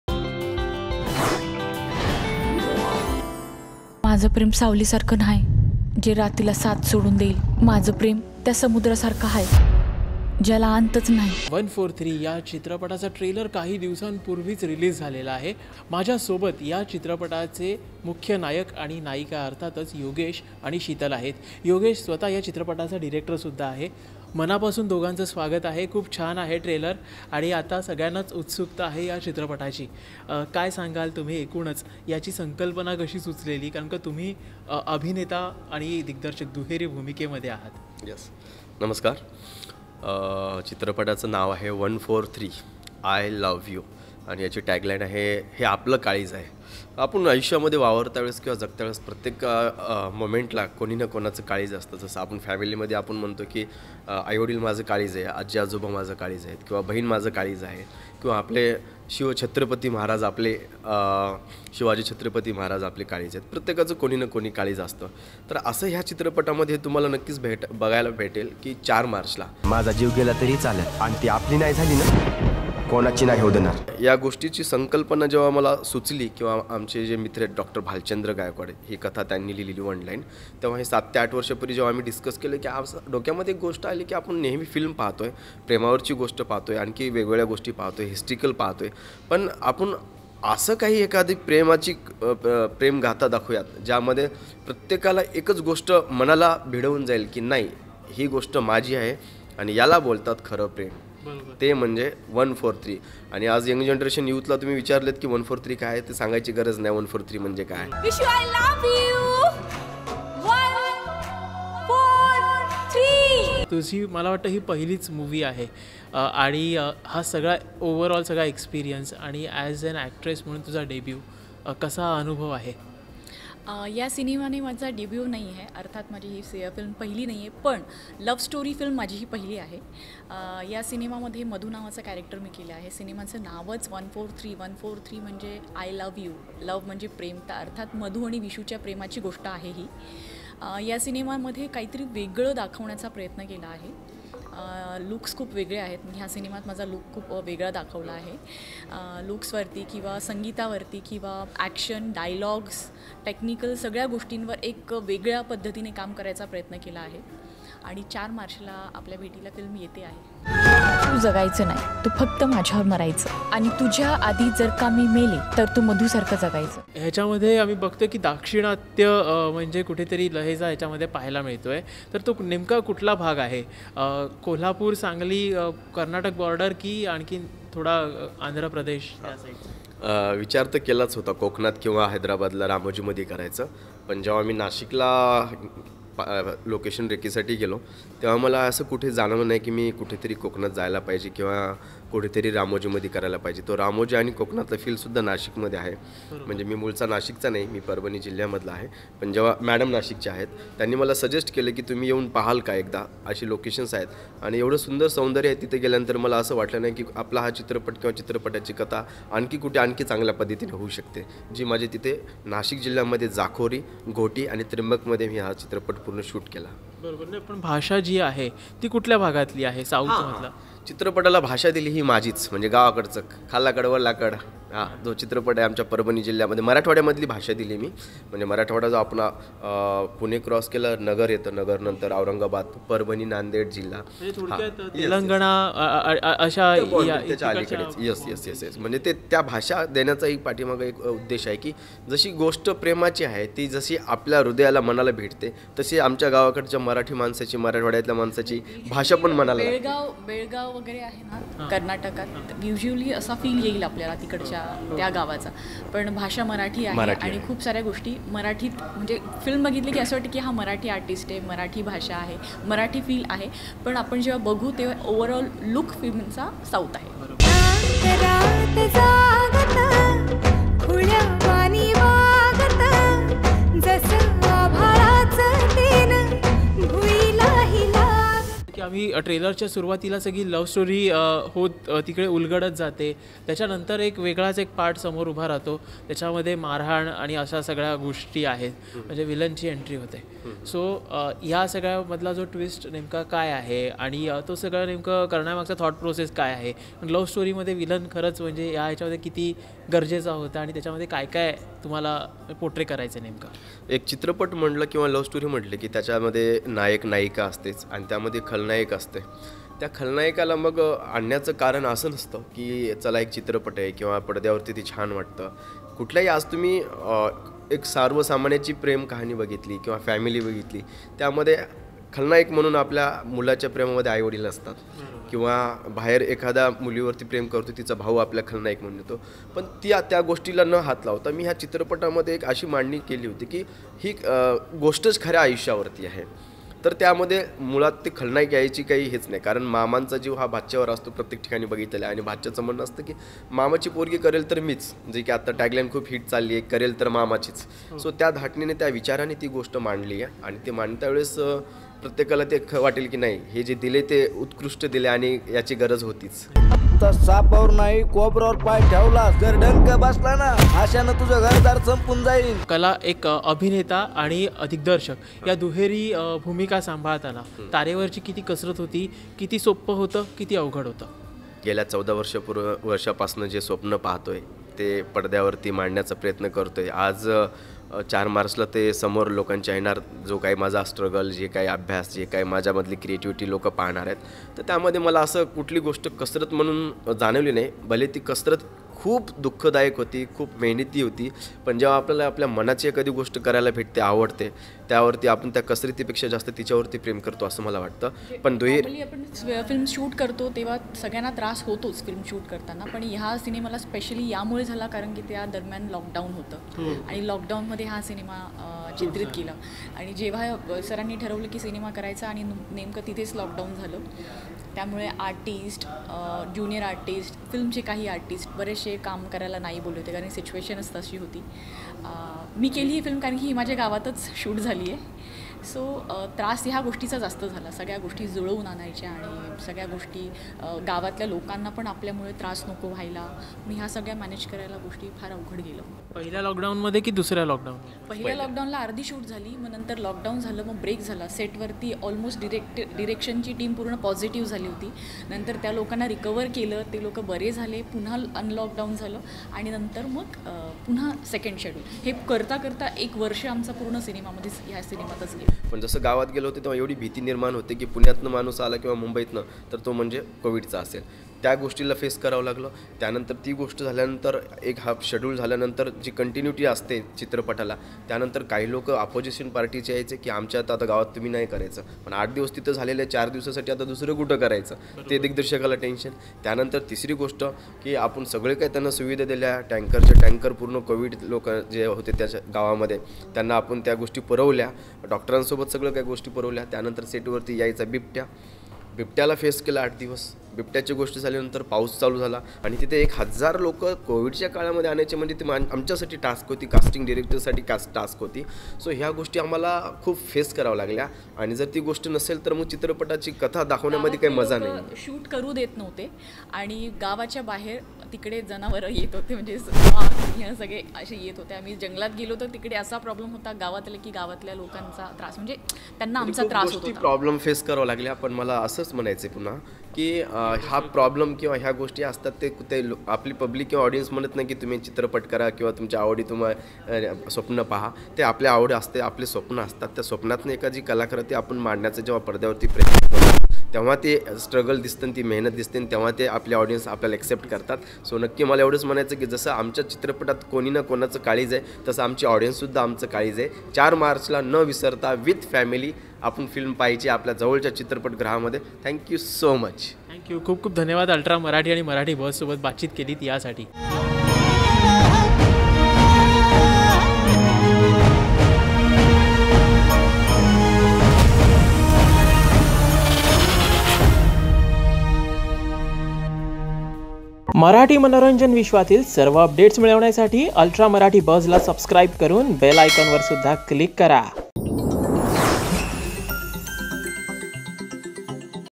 143 ( वन फोर थ्री ) |चित्रपटाचे तंत्रज्ञ, कलाकार कोरोना बाधित झाले होते | Yogesh & Sheetal Interview
143 ( वन फोर थ्री ) | चित्रपटाचे तंत्रज्ञ, कलाकार कोरोना बाधित झाले होते | Yogesh & Sheetal Interview | Release Date 4th March

"143" चित्रपटाचा अभिनेता आणि दिग्दर्शक योगेश भोसले यासह अभिनेत्री शीतल अहिरराव यांनी "अल्ट्रा मराठी बज" सोबत साधलेला संवाद पाहणार आहोत ! Interview By Sagar Maladkar Cameraman - Gaurav Borse Video Editor - Ajay Shukla